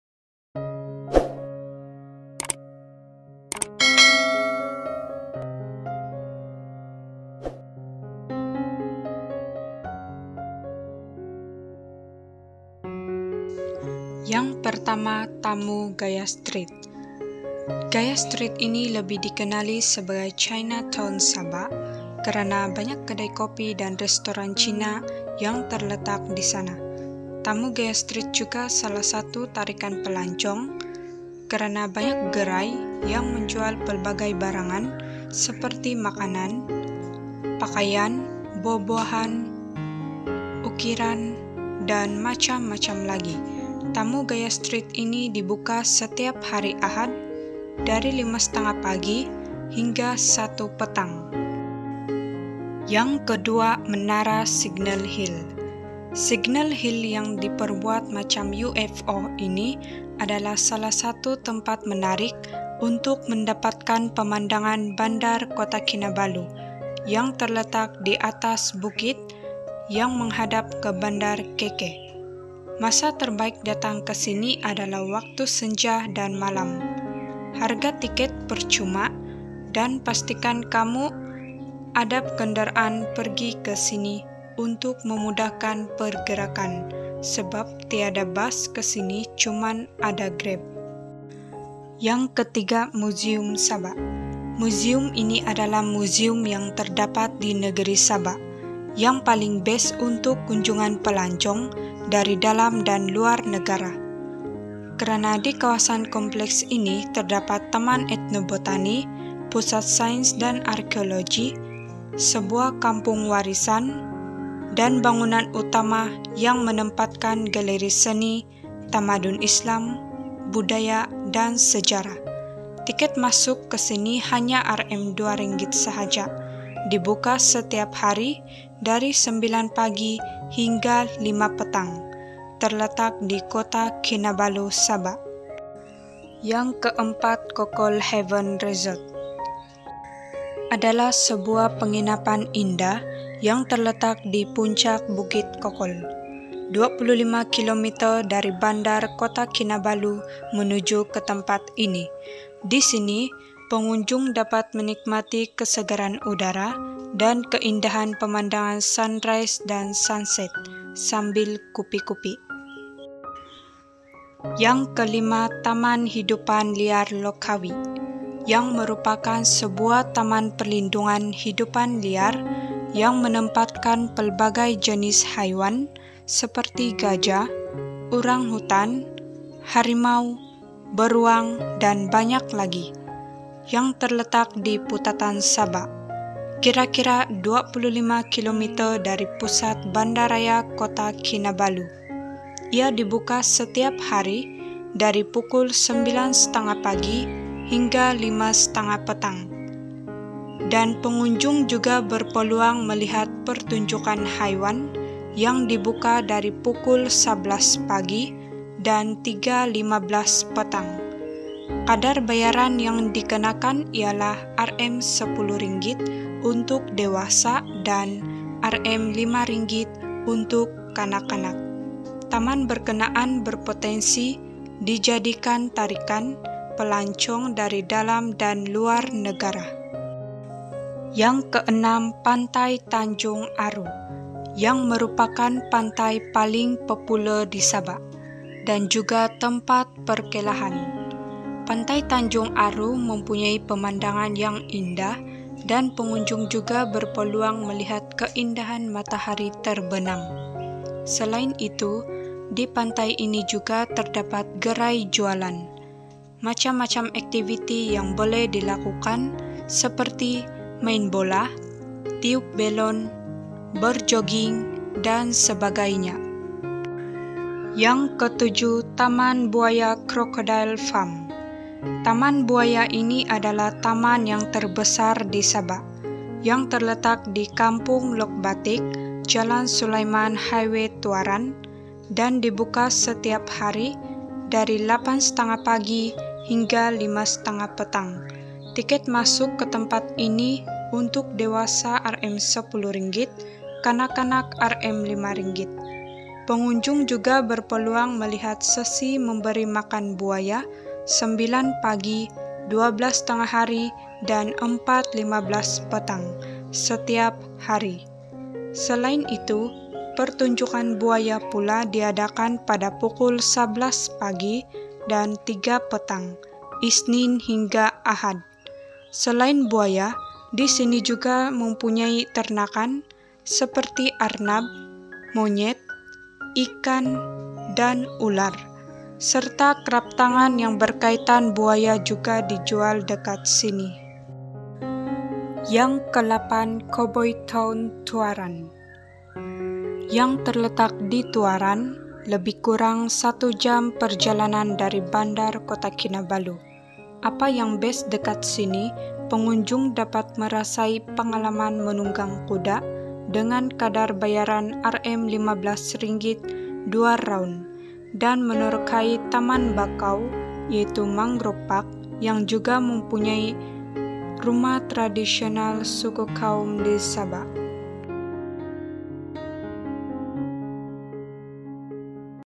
8 tempat menarik yang harus dilawati di kota kinabalu sabah.part 1
Banyak lagi tempat yang menarik di kota kinabalu yang harus dilawati tetapi setakat ini list yang pernah Saya kunjungi   sebelumnya.jika ingin berkunjung ke tempat ini bawalah keluarga atau rakan2 kamu bagi merasai pengalaman sendiri.

ok guys itu sahaja sedikit ulasan Saya mengenai 8 tempat menarik yang harus dilawati di kota kinabalu dan semoga video ini bermanfaat.